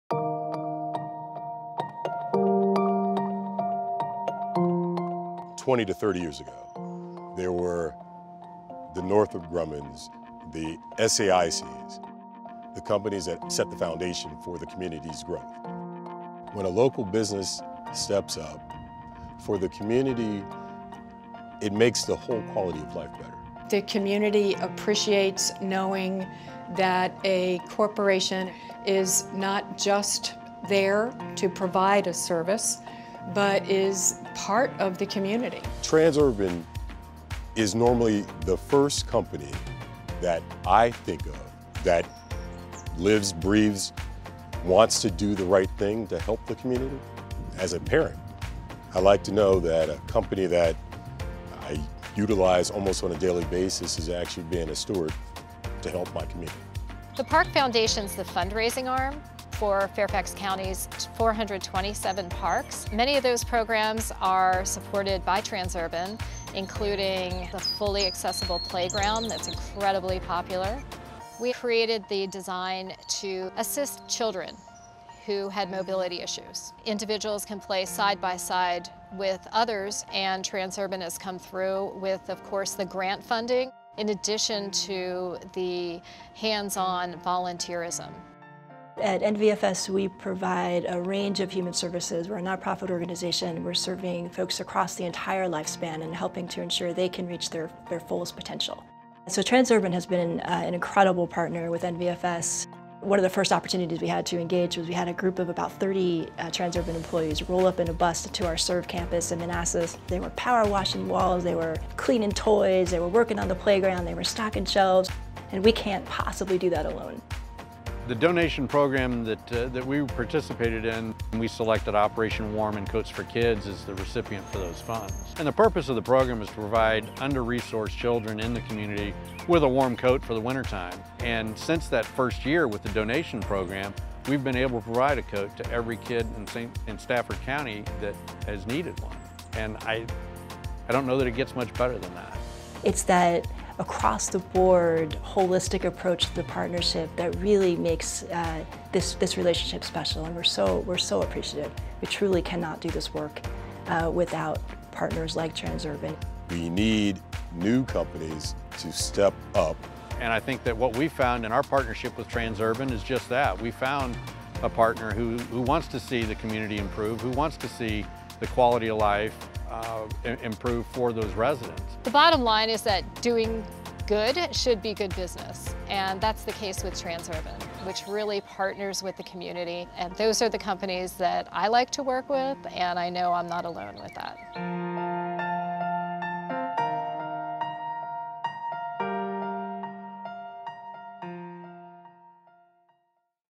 Transurban in the Community
Transurban, the operator of the 495 and 95 Express Lanes, is committed to being a good partner to the communities in which we operate.